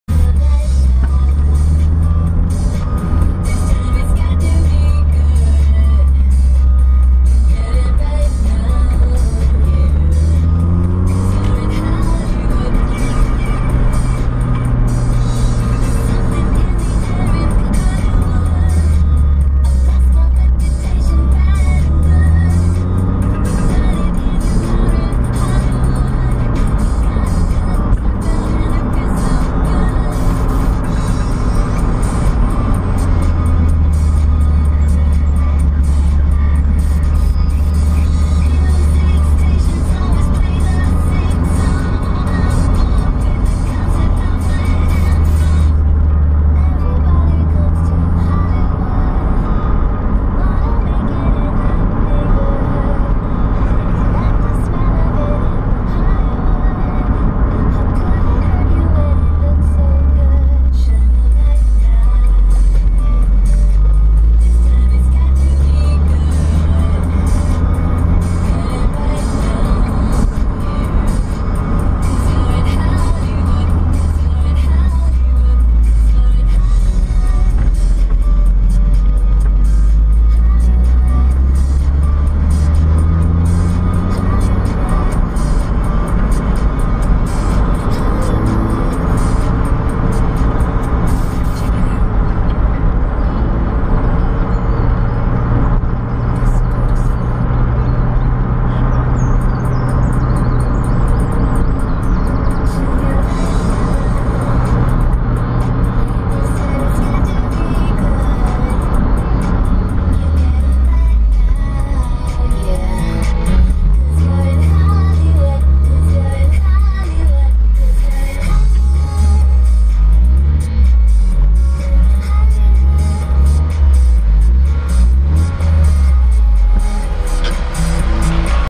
mx5 down col de la madeleine
first part of the run down the col de la madeleine